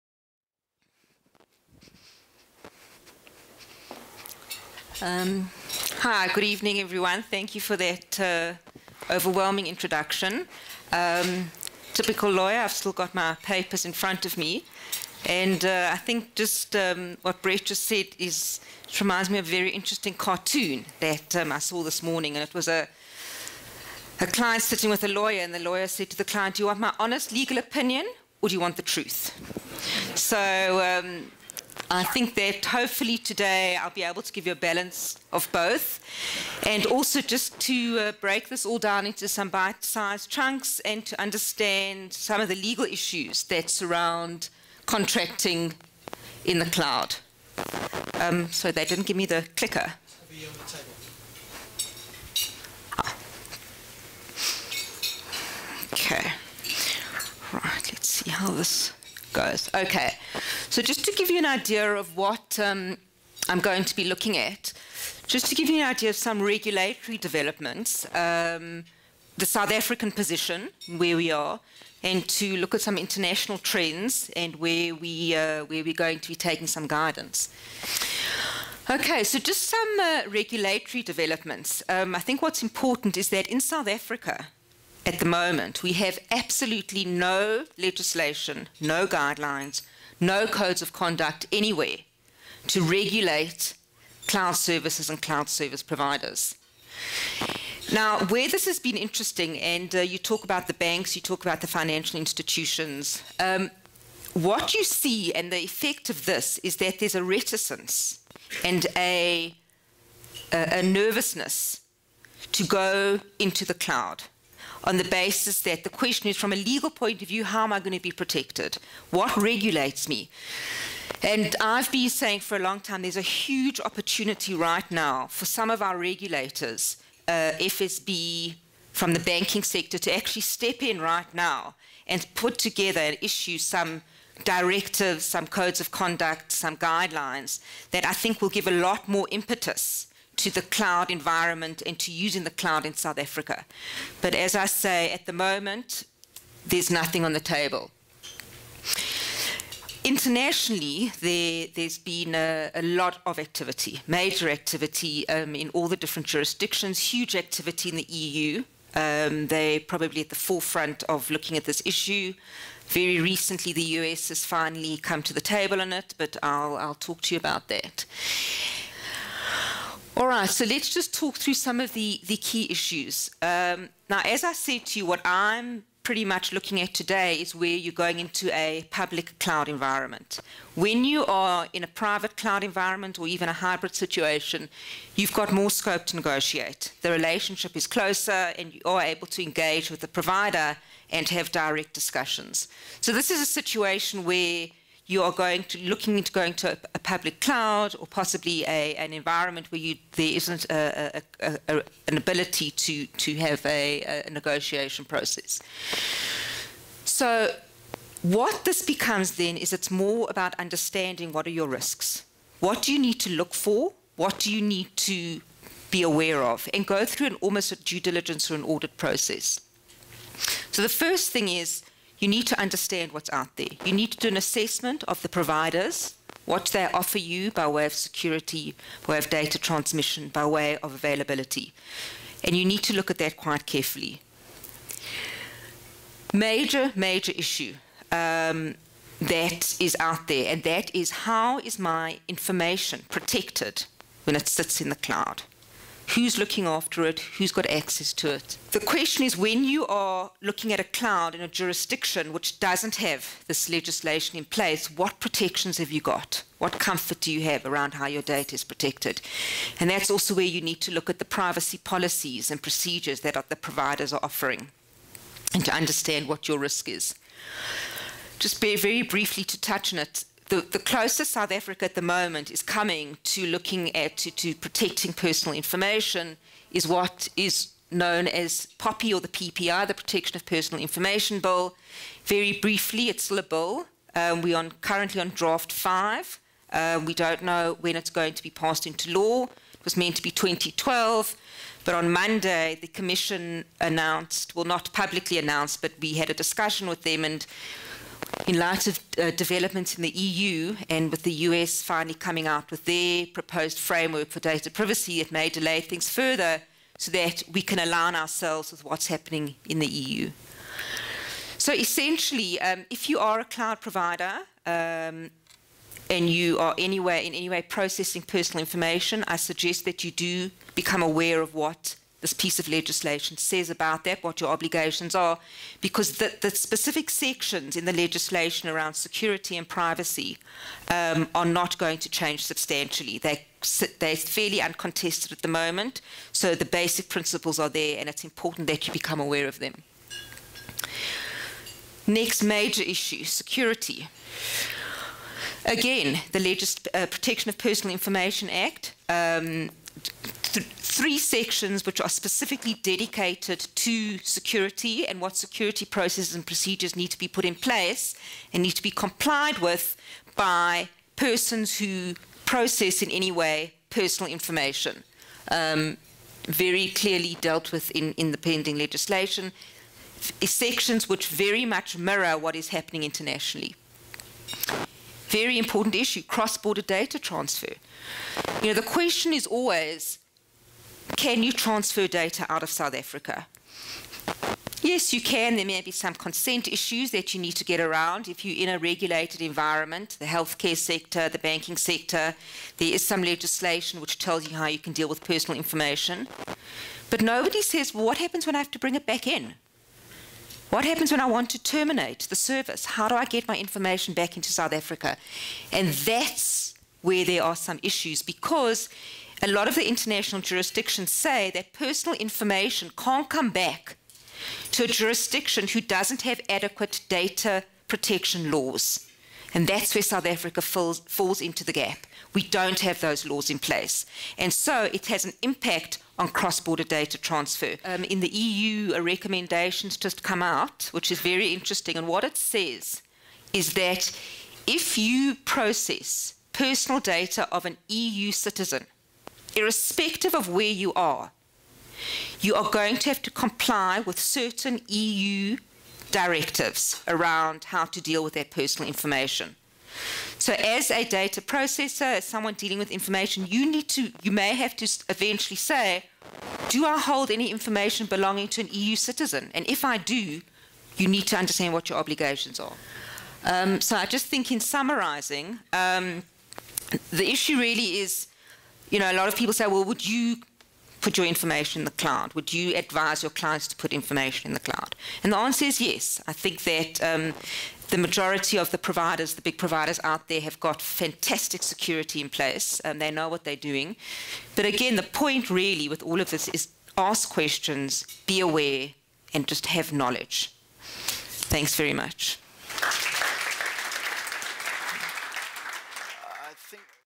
Tammy Bortz on Contracting in the Cloud_15032012.wmv
is a Director at Werksmans Inc a leading South African law firm and heads the technology law department.
Tammy's clients include London FTSE top ten, JSE listed, global and local private companies. Matters include advising two of the four major South African Banks, representing JSE listed companies on major outsourcing transactions, due diligence investigations of various businesses to assess technology and regulatory exposure, advising on electronic payment and invoicing transactions and on electronic draw card and payment initiatives.